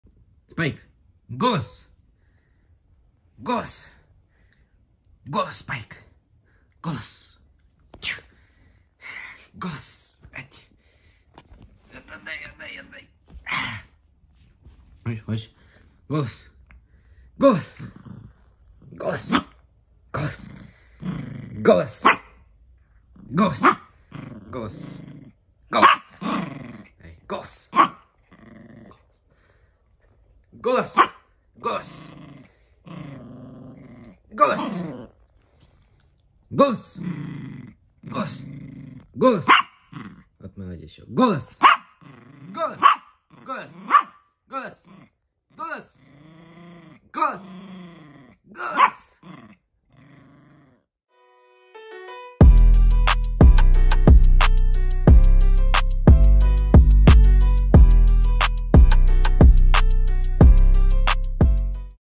JUST LOOK: This Dog is SO ANGRY!
Hello missis and misters. It is my first video. I hope you liked it.

Spike is a very obedient dog. If it seemed to you that a person is playing cruelly with him, then this is not so. Spike loves to be played with like that, and Spike himself likes to bite his fingers often)